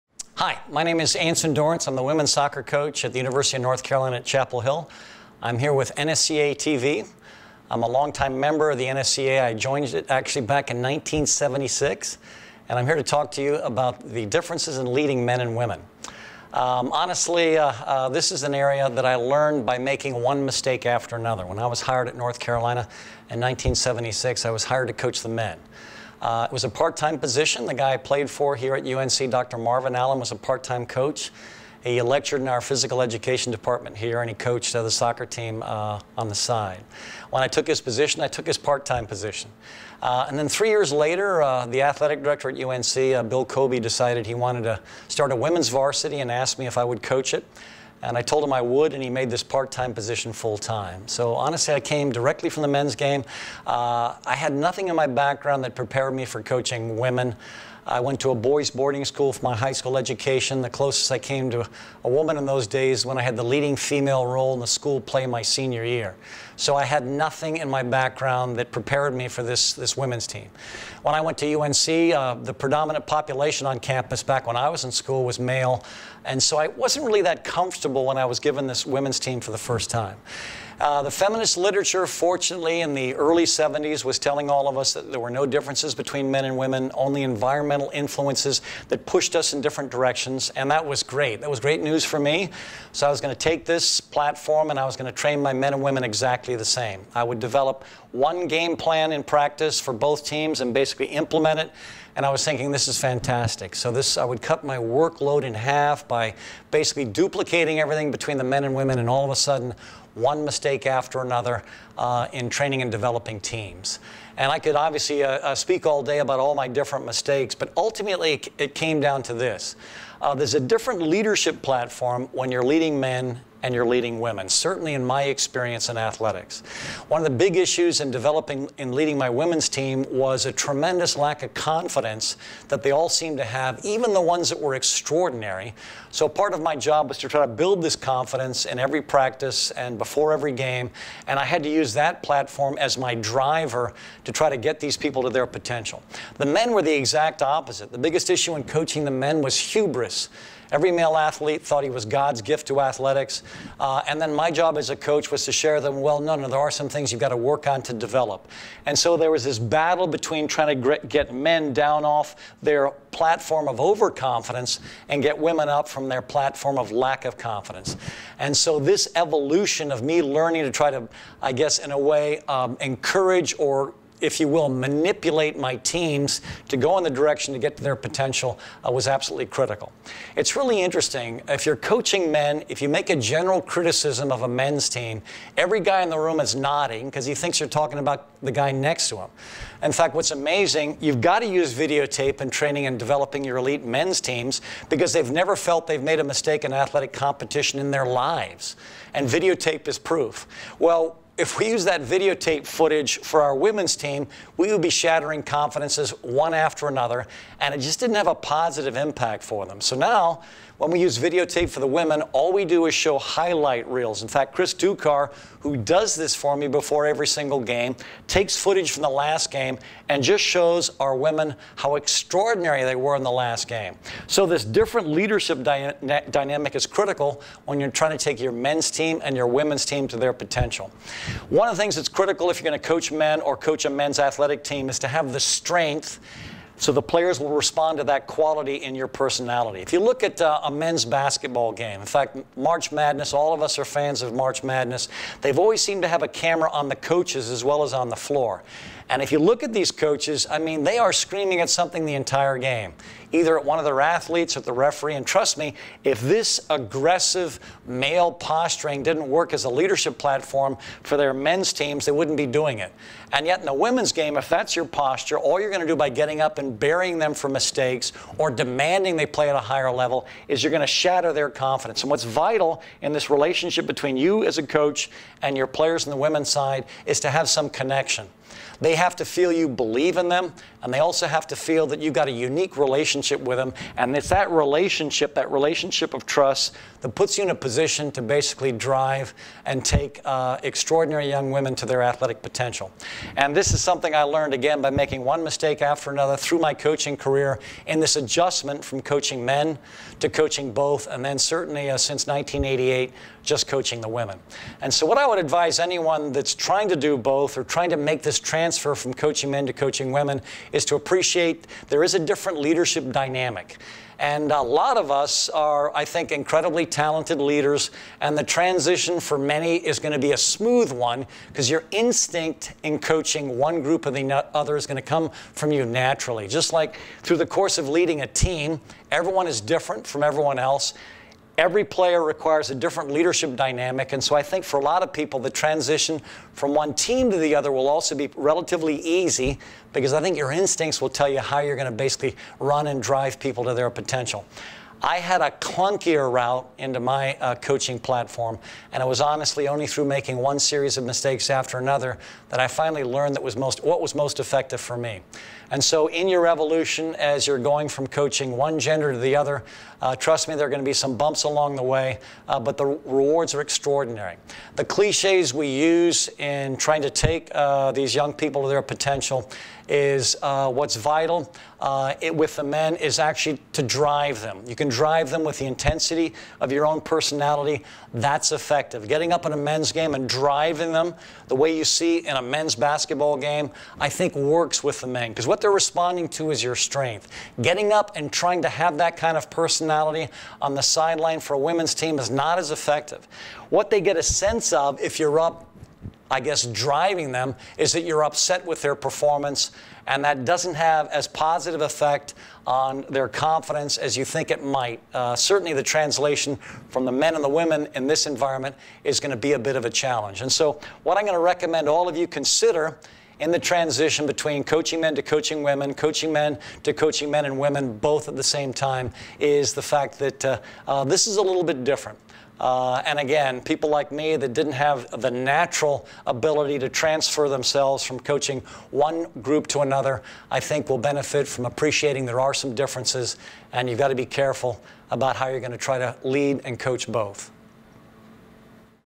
UNC's Anson Dorrance on Coaching Men and Women
Anson Dorrance, NSCAA member since 1976, is one of the most legendary collegiate soccer coaches of all time. He started his coaching career by coaching both the women's and men's soccer teams at the UNC at Chapel Hill (UNC).

Coach Dorrance discovered early the difference between leading women and men soccer players and he shares that discovery with NSCAA members in this special edition video on NSCAATV.